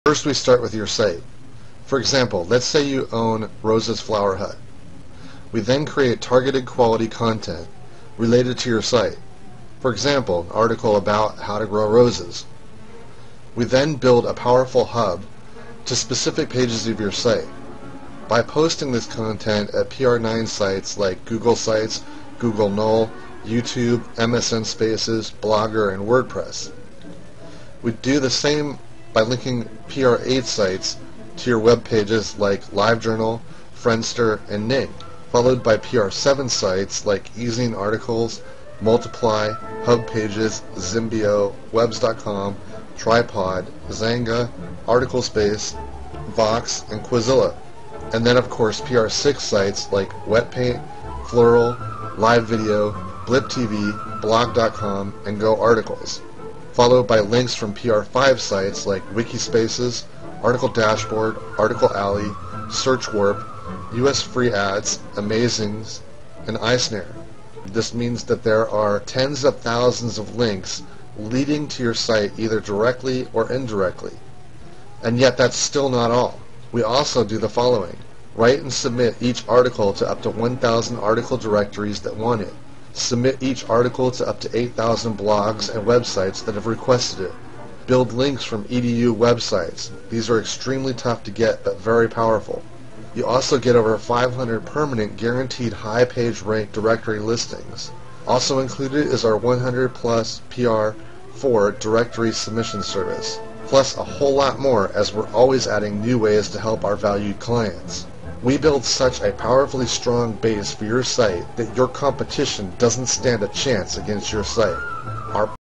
Search Engine Ranking -- 7 Steps To Get Rank On First Page Of Google
Below is my post about search engine ranking tips. I have taken each of the key points and provided my insight for 7 steps to get top rank in Google search. You can read the full Search Engine Ranking